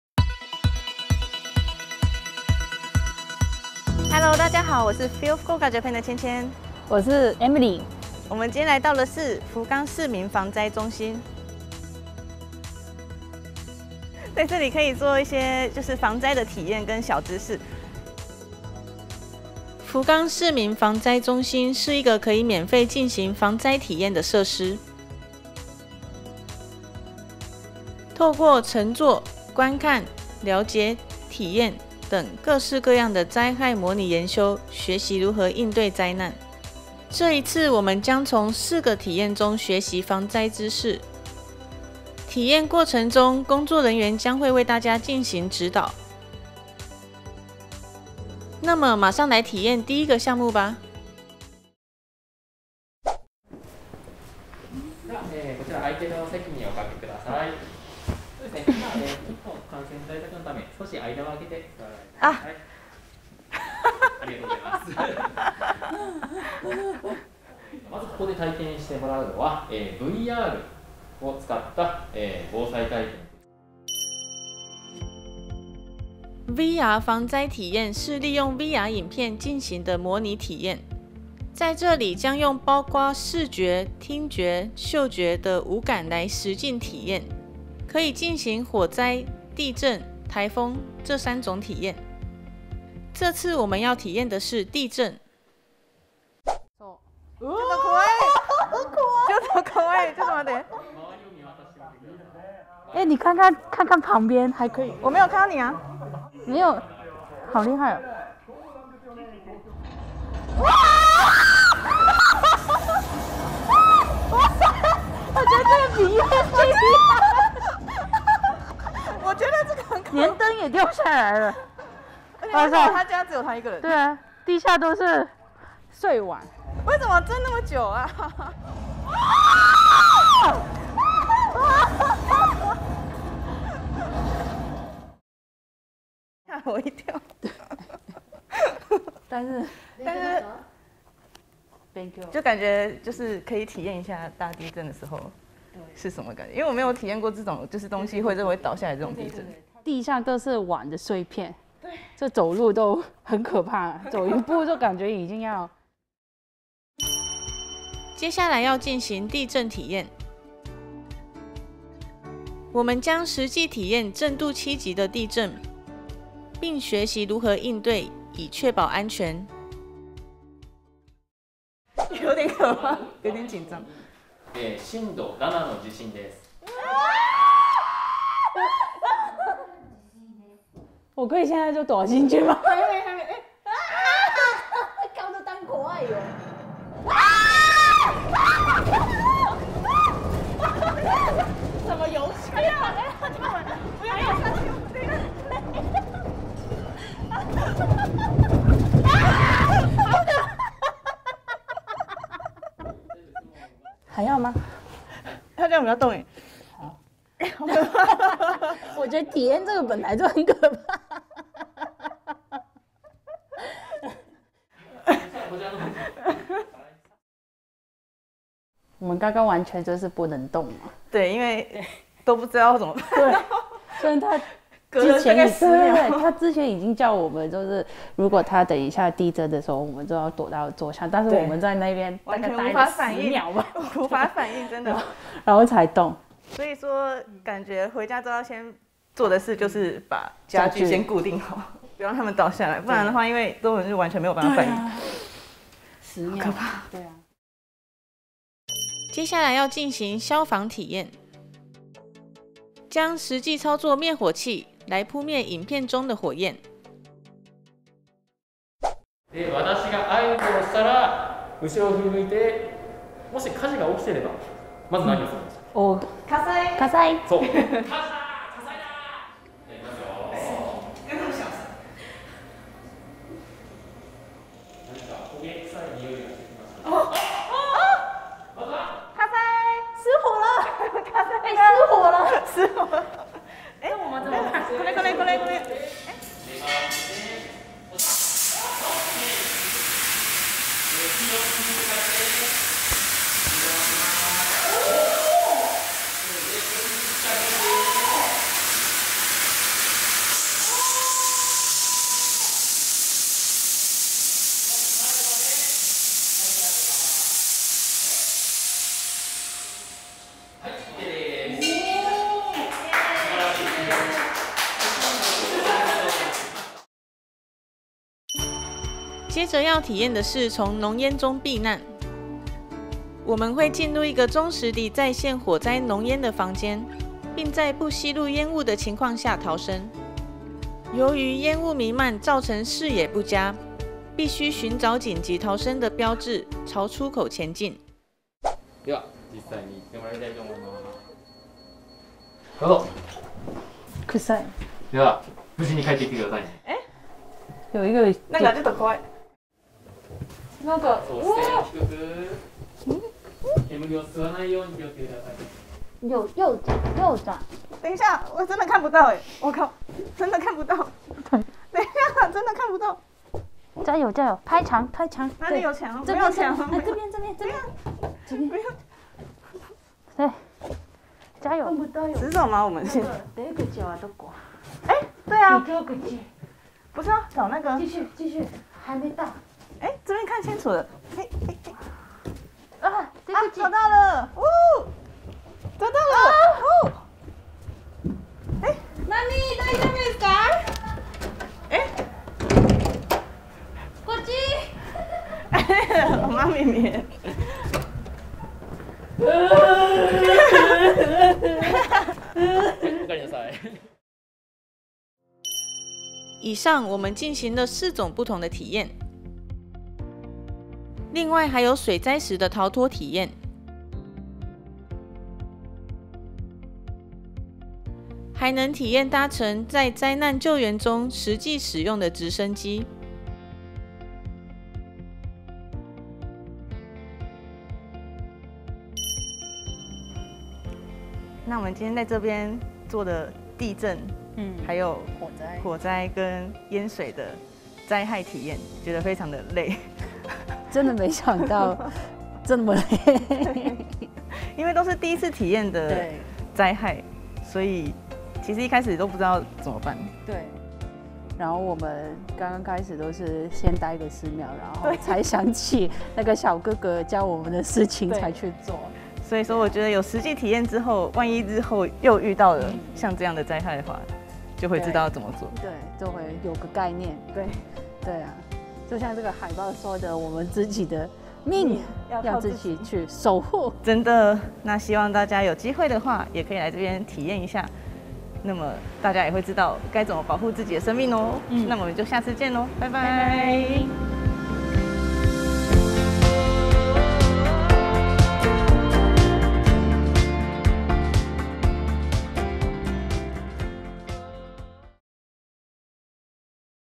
Be prepared in the event of a natural disaster with these emergency drills!
One thing that many people from other countries find surprising when they come to live in Japan is how many different natural disasters the country has. Many people don’t know what to do in the event of an emergency and start to panic. That’s why we recommend preparing yourself for an emergency situation by practicing with these emergency drills! Today we visited the Fukuoka Citizens’ Disaster Prevention Center where you can experience a variety of different emergency drills so that you’ll be ready to act in the event of a natural disaster such as an earthquake or fire.